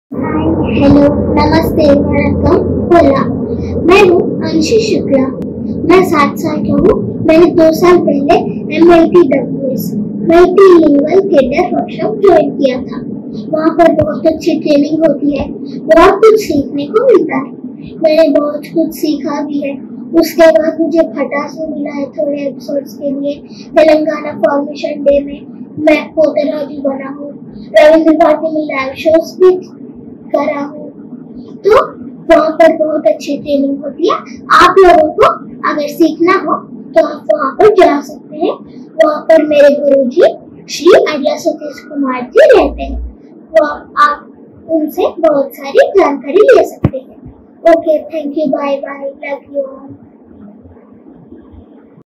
Testimonial by Patas Shukla for Stage 2 Screen Multilingual Acting Workshop 2019
Digi7TV Presents Stage2ScreenActingWorkshop 2019, a Multilingual Theatre and Media Workshop by AdlaSatishKumar with DigiquestAcademy to Help Aspiring Actors to Fine Tune the Tools of an Actor Learn the Craft and Apply onto Various Mediums.